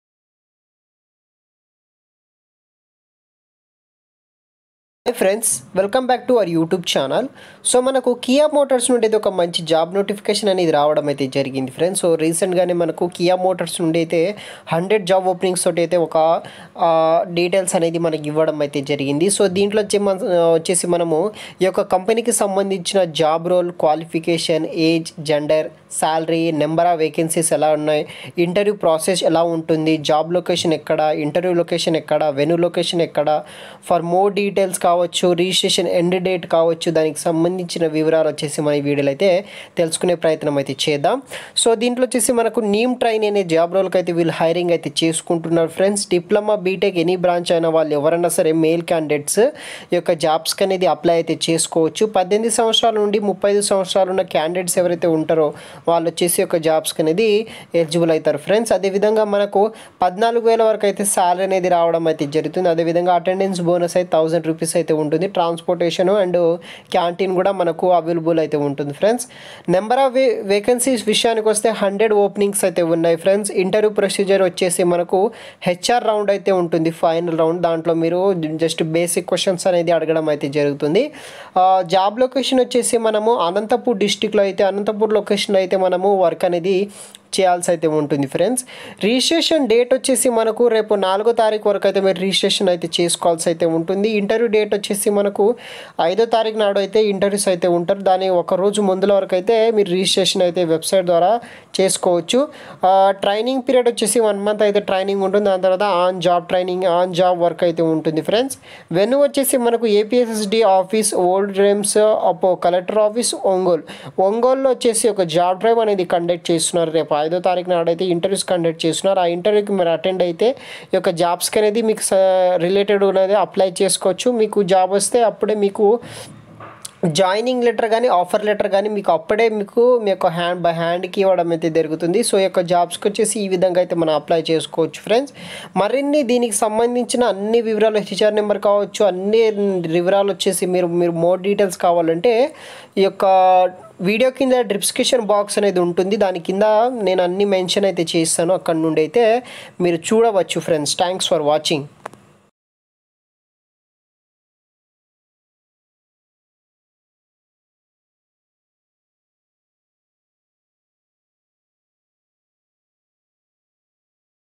Kia Motors Manufacturing Company Jobs In Telugu 2022 || Latest Urgent Job Recruitment In Telugu 2022
➡️Recruitment Drive Details 📌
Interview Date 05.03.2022
Training Period : 01 Month
Venue:
APSSDC Office,Old rims,
Opp: Collector Office,Ongole.

➡️ Telegram Gruop Link Join Now🔥